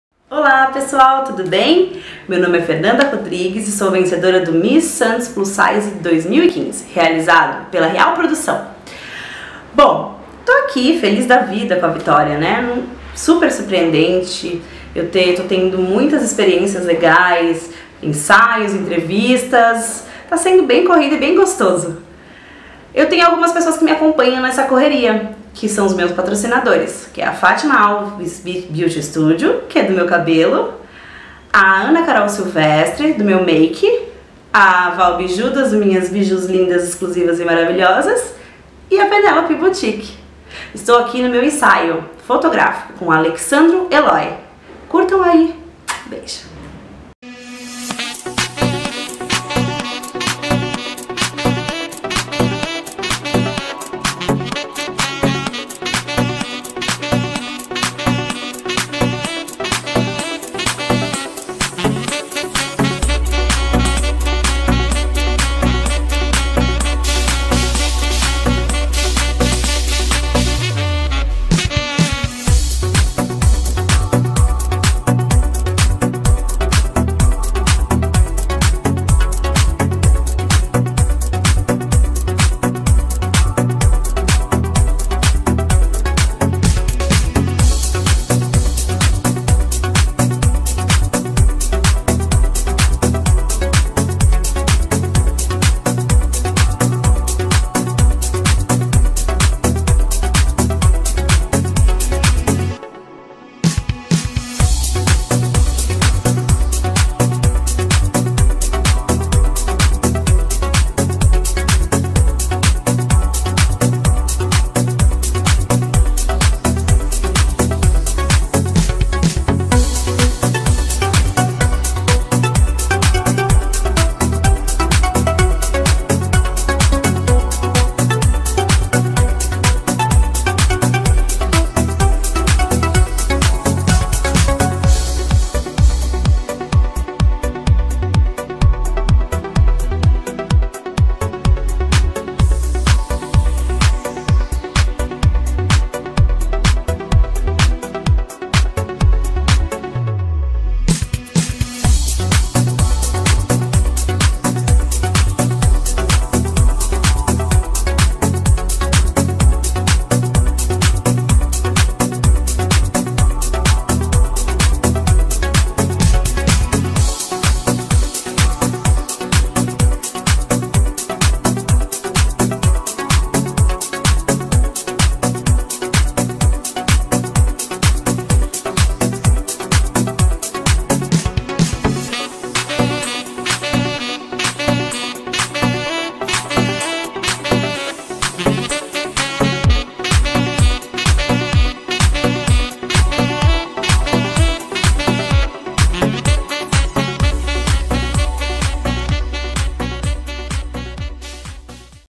Fernanda Rodrigues Ensaio fotográfico
Fernanda Rodrigues a Miss Santos Plus Size 2015, realizou no dia 09 de abril um lindo ensaio com Alexsandro Eloi da Eloi - Photo & Film !!! Confira !!!